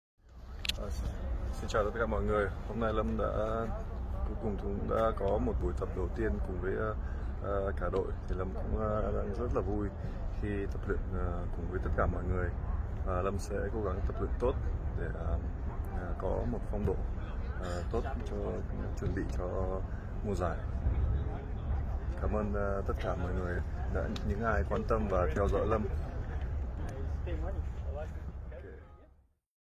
Đặng Văn Lâm gửi lời chào sau buổi tập đầu tiên cùng Cerezo Osaka
Sau buổi tập đầu tiên với Cerezo Osaka, Văn Lâm đang rất nóng lòng được cùng toàn đội hướng tới những mục tiêu đang chờ đón của mùa giải